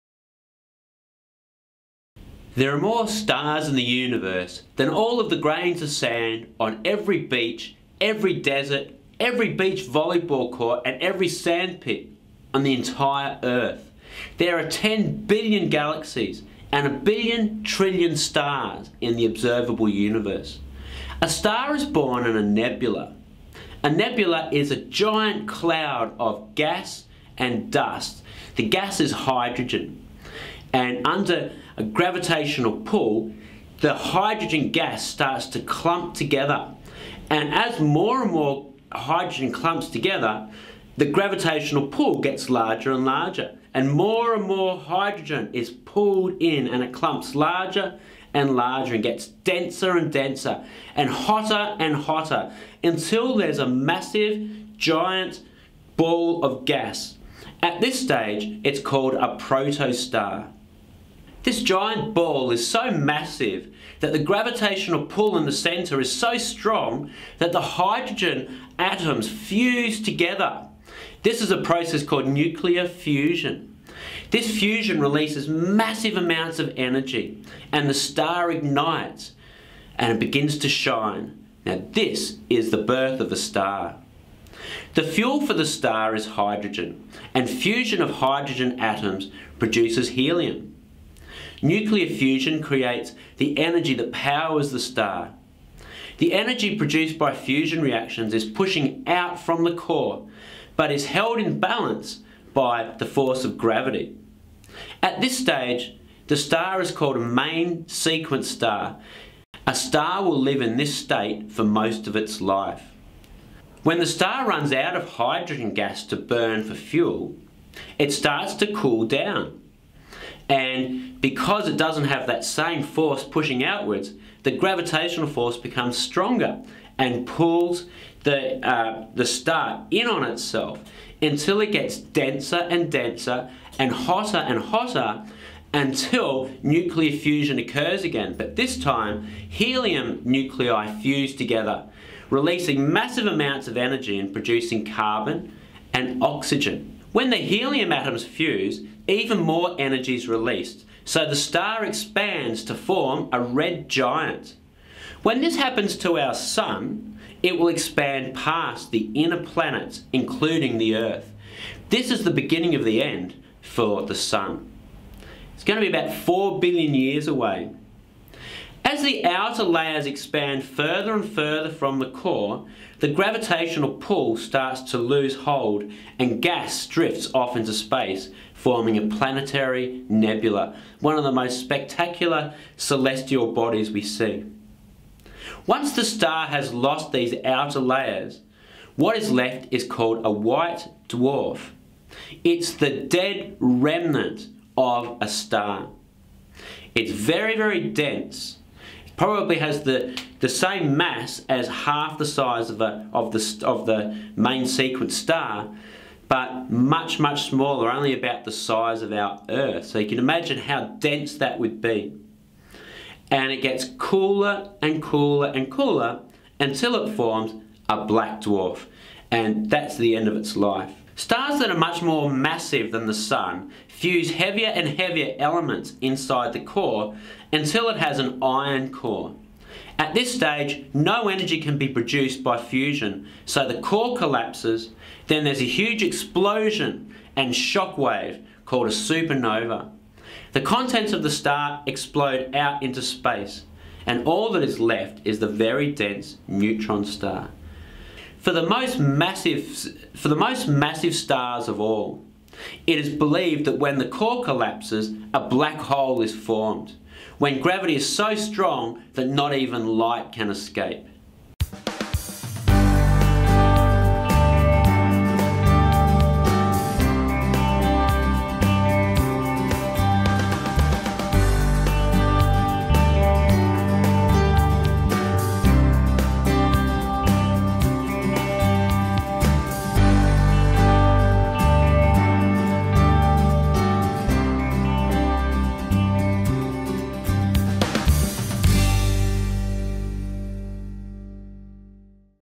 Life cycle of stars
The fate of a star depends on its size.  This video will explain how a star is born and what happens at the end of a star's life.